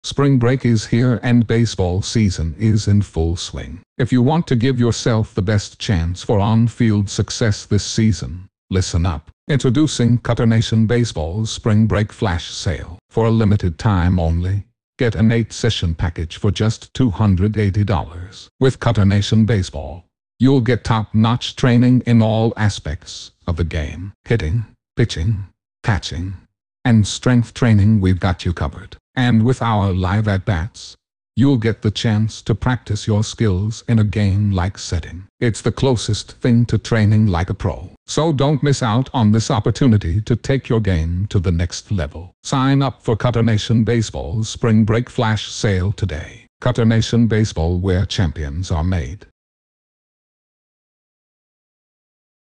Spring break sale now live! Use code: SpringBreak23
Watch as Pitching Ninja, Kyle Boddy, and Cutternation bring you some of the best live at-bats from Dallas, Texas! From viral curveballs to TikTok sports, this video has it all and guarantees to rack up views. Get ready to witness some of the biggest hits and impressive pitches in Texas!

Marc Pro for 10% OFF
CNBSB
Clean Fuego for 10% OFF:
CutterNation
AXIO Trainer